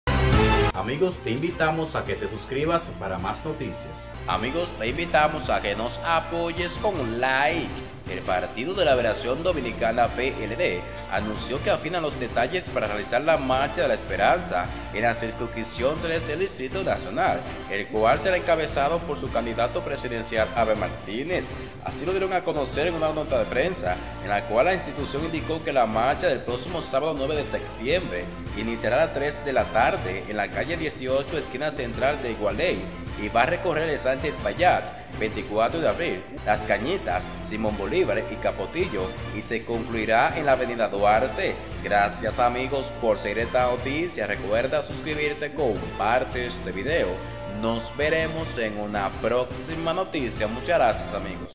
MIRA LO QUE ANUNCIO EL PARTIDO DE LA LIBERACIÓN DOMINICANA PLD
El Partido de Liberación Dominicana (PLD) anunció este jueves que afinan los detalles para realizar la Marcha de la Esperanza en la circunscripción tres del Distrito Nacional, el cual será encabezado por su candidato presidencial, Abel Martínez.

Así lo dieron a conocer en una nota de prensa, en la cual la institución indicó que la marcha del próximo sábado 9 de septiembre iniciará a las 3:00 de la tarde, en la calle 18 esquina Central en Gualey y recorrerá el ensanche Espaillat, 24 de abril, Las Cañitas, Simón Bolívar y Capotillo y concluirá en la avenida Duarte.

La organización política agregó, que desde el pasado domingo la actividad es promovida mediante visita casa por casa y el uso de promociones audiovisuales en las redes sociales y perifoneo por las calles de las barriadas de la zona norte de la capital.